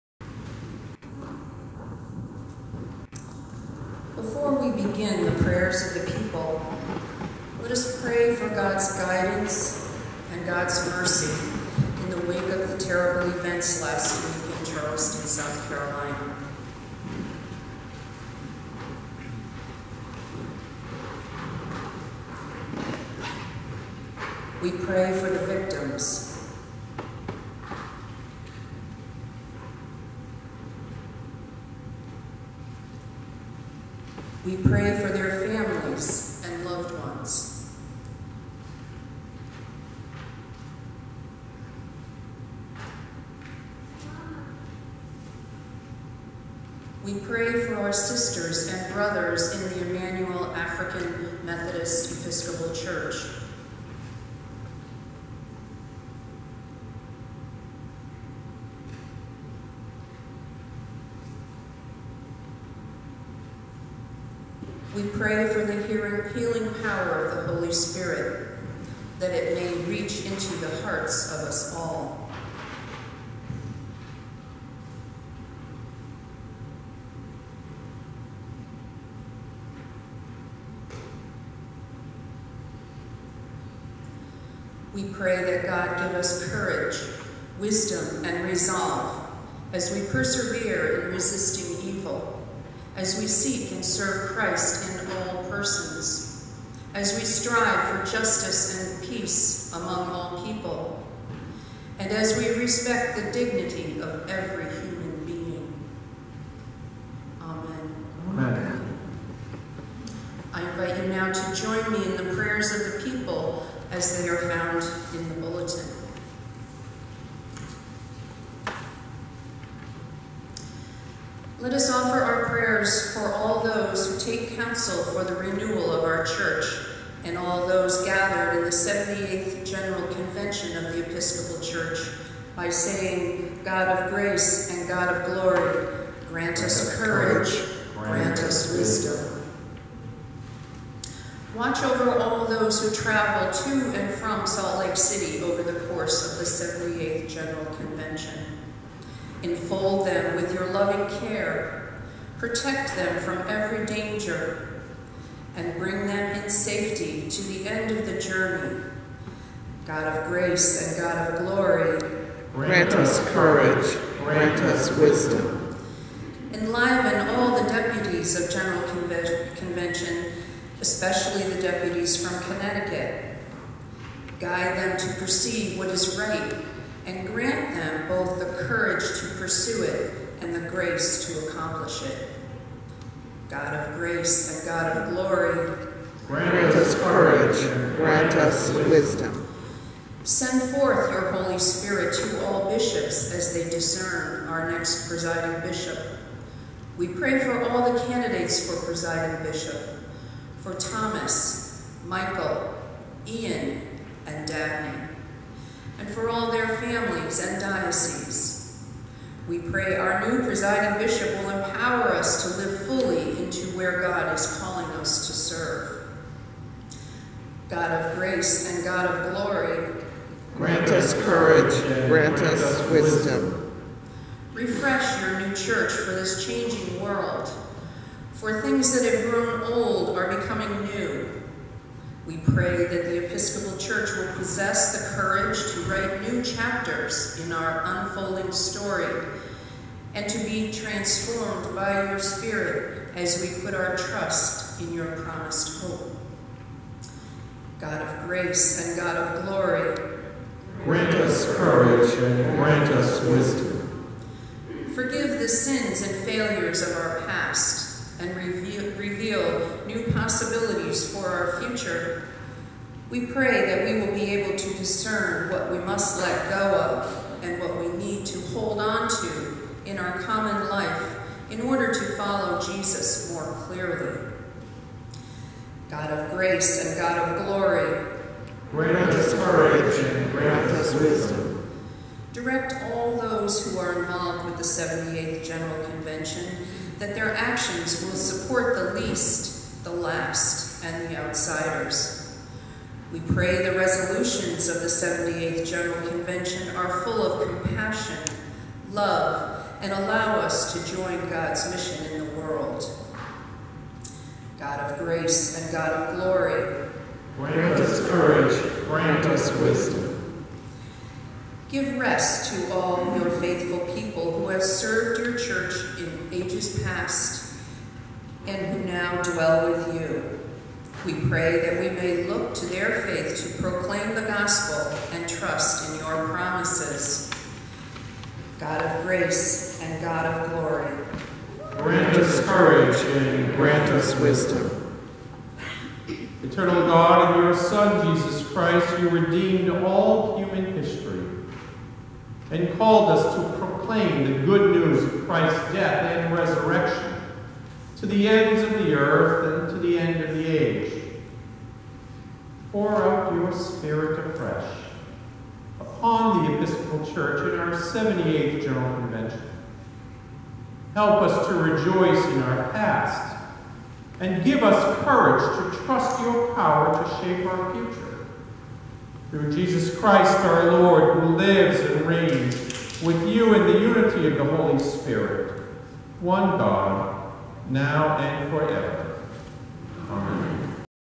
Prayers for Charleston, and the 78th General Convention
Kim Polhemus, Intercessor
June 21, 2015
St. John's Episcopal Church, New Milford, CT